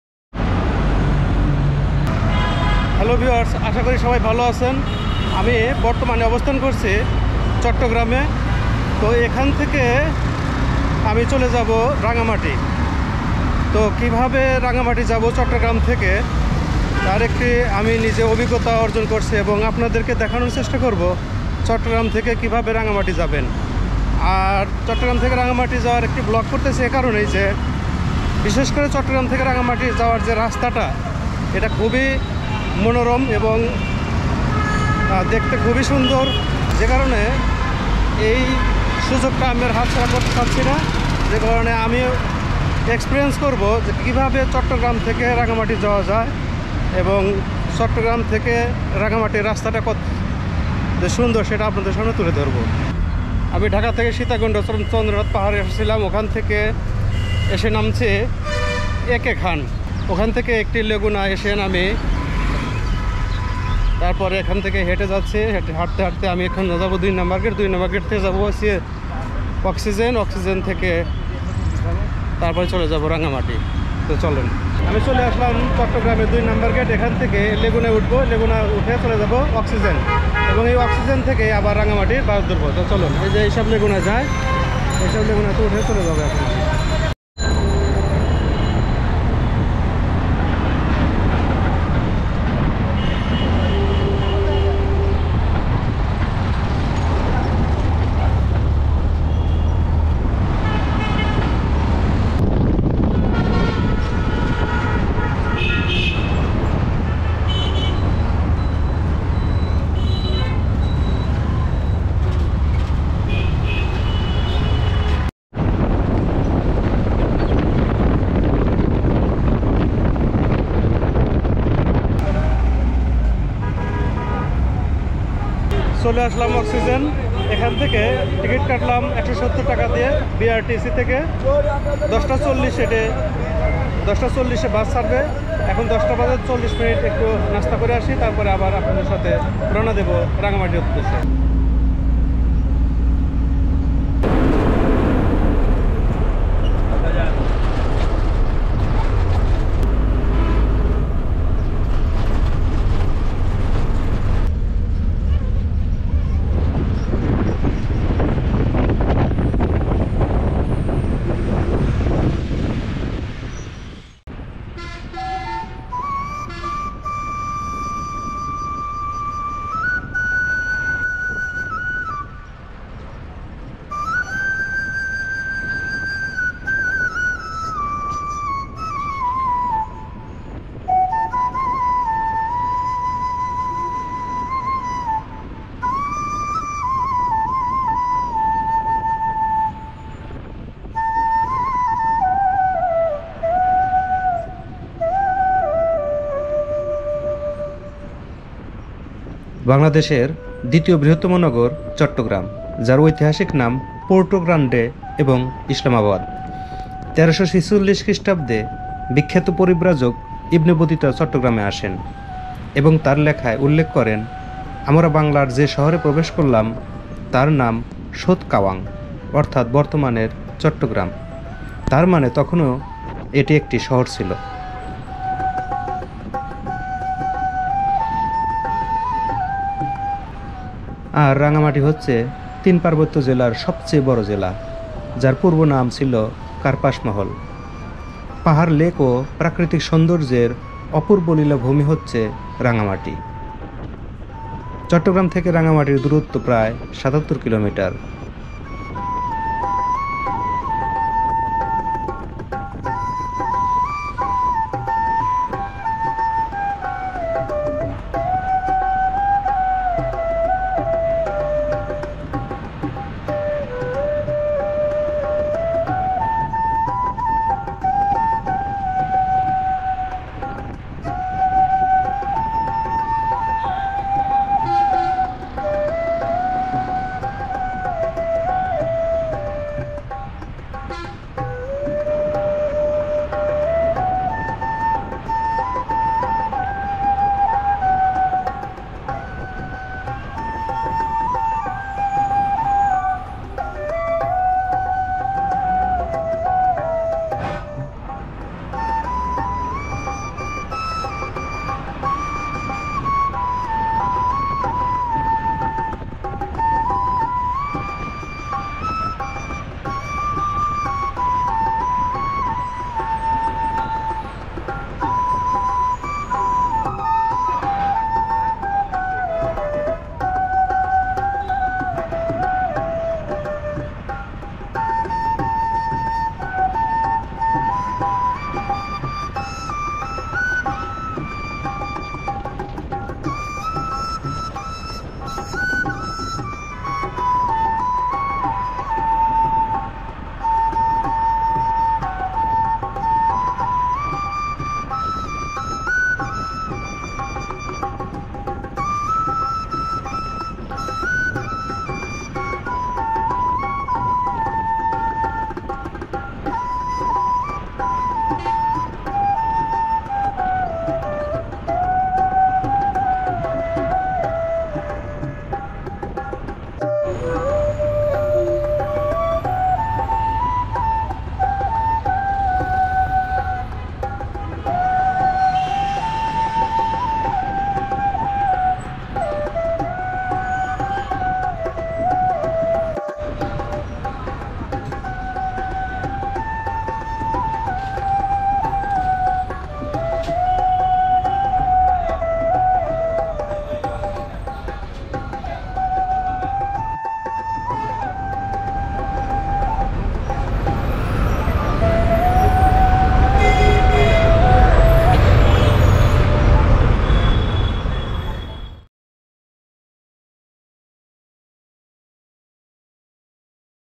CHATTOGRAM TO RANGAMATI TOUR.
চট্টগ্রাম থেকে রাঙ্গামাটি কিভাবে যাবেন তার একটি ভ্রমন গাইড।